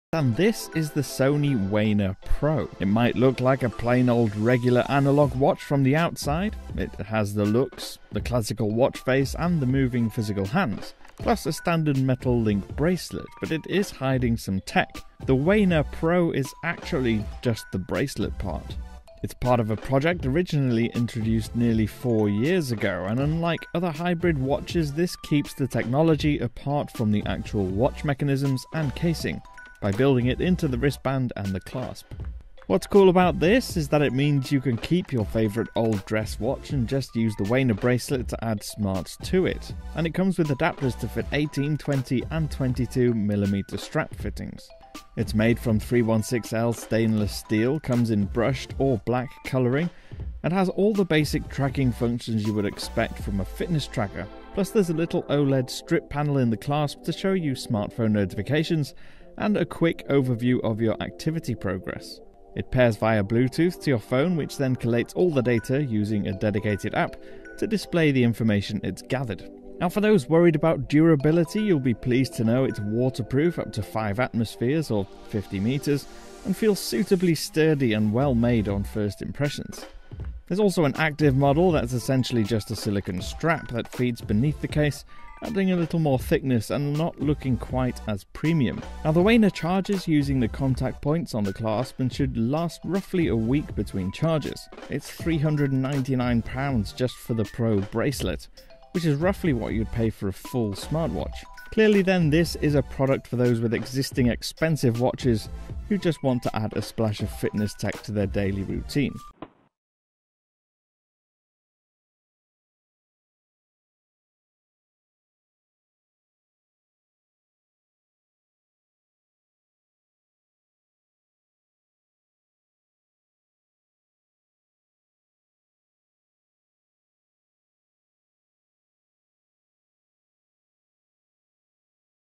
Sony Wena strap turns any watch into a smartwatch
The Sony Wena Wrist is a smart strap designed to be worn with existing mechanical watch faces. You can use it to turn any classic or fashion watch into a smart bit of kit.

The tiny display is built into a buckle, showing phone alerts and basic activity tracking, and the strap also handles contactless payments. It's water resistant to 50 meters, and if you're after the complete package, Sony has also developed five matching watch faces with mechanical, solar and quartz movements.